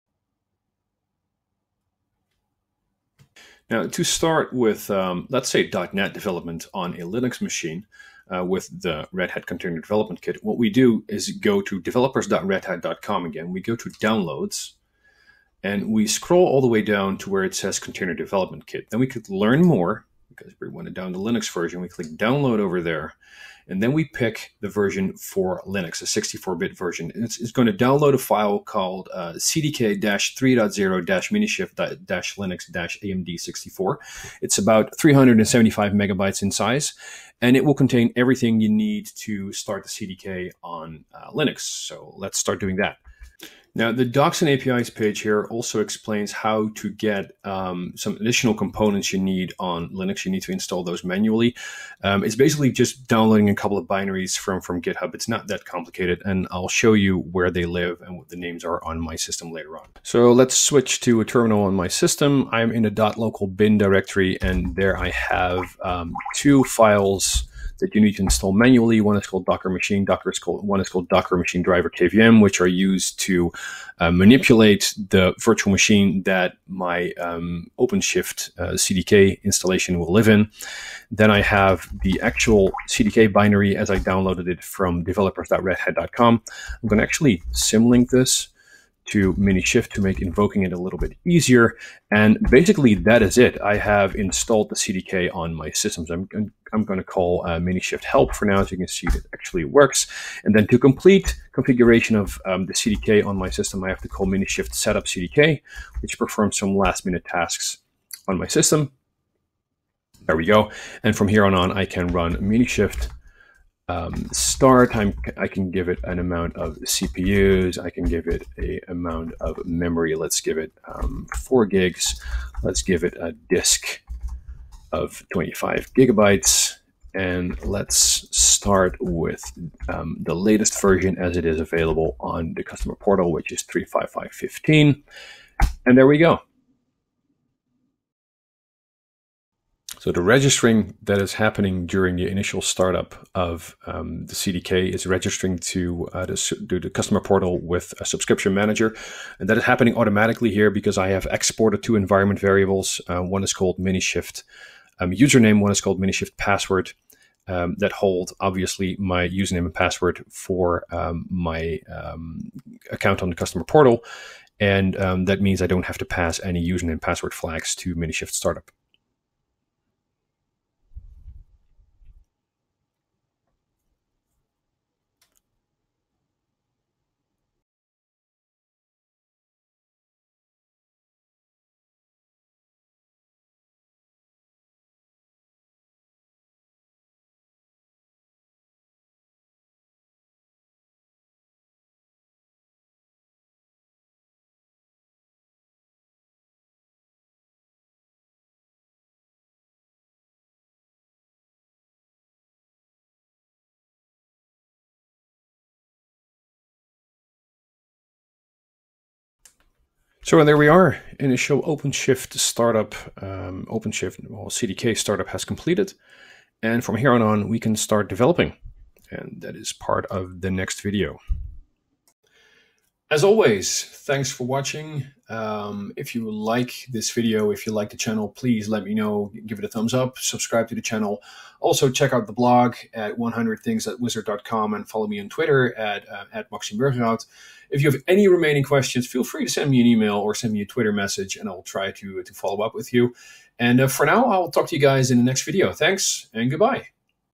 Installing the CDK on Linux
In this video, I walk you through installing the Red Hat Container Development Kit (CDK) on a Linux machine. The machine I use is a Fedora 25 laptop, but this procedure should work equally well on any other flavor of Linux.

This video is a recordings of one of the demos from my presentation on the Container Development Kit on the Red Hat Tech Day in Utrecht, NL on June 13th, 2017.